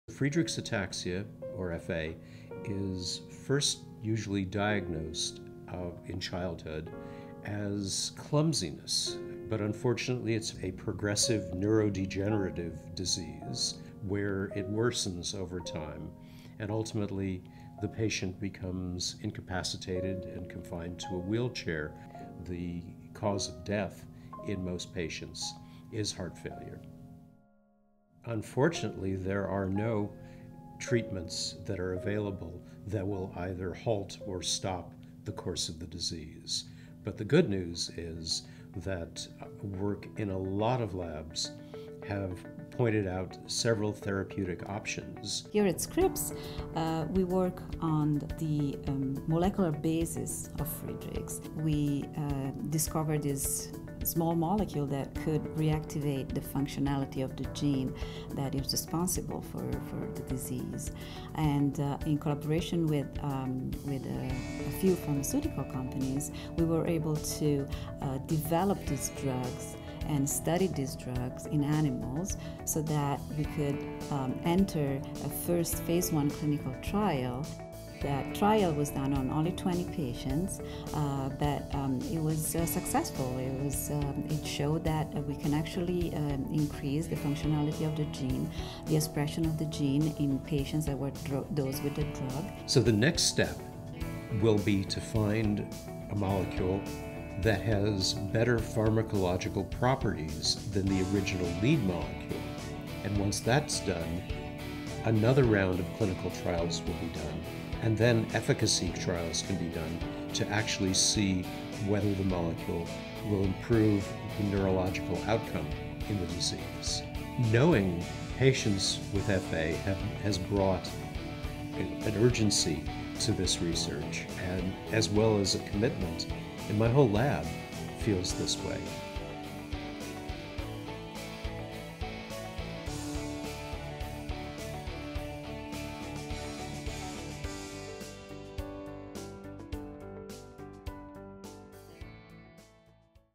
Taking on Friedreich's Ataxia
Scientists at The Scripps Research Institute (TSRI) are working on a therapeutic to treat Friedreich's ataxia, a progressive neurodegenerative disease.

Click [CC] in video viewer to access Closed Captions and Subtitles.